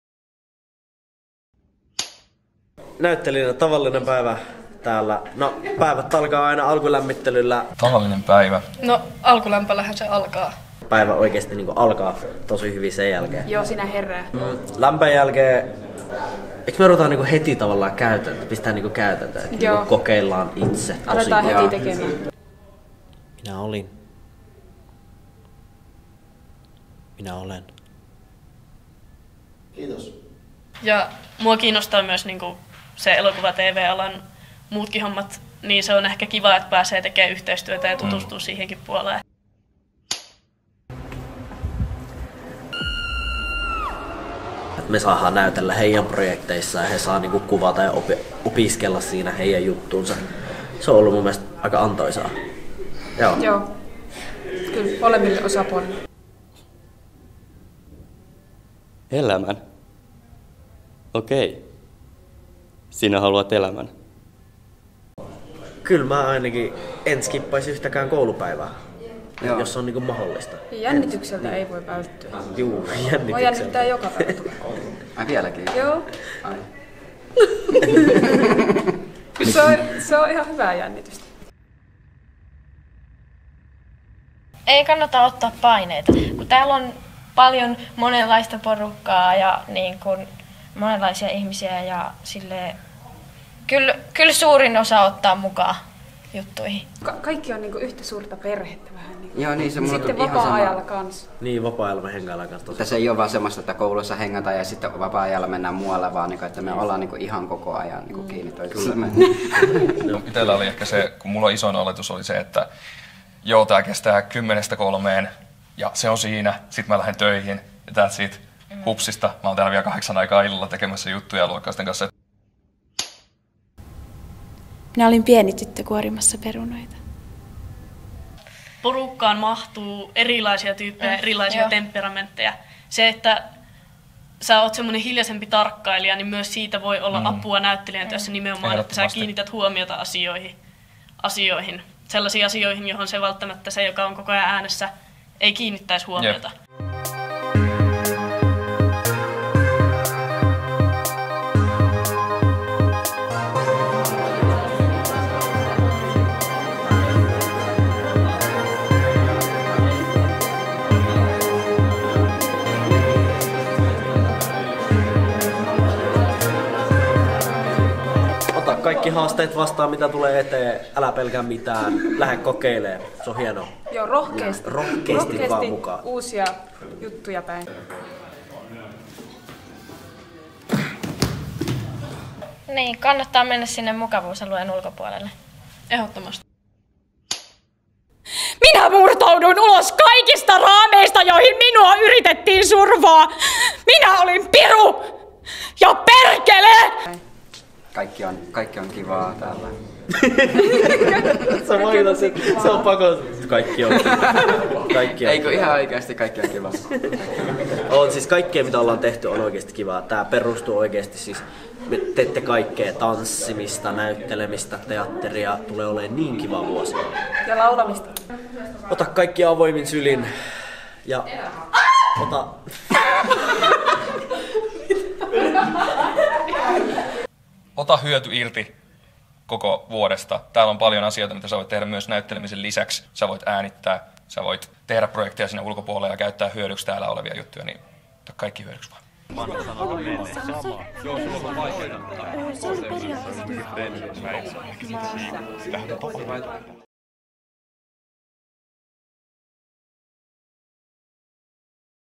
Näyttelijälinjan esittely 2024
Voionmaan koulutuskeskuksen näyttelijälinjan 23-24 opiskelijat kertovat opinnoistaan.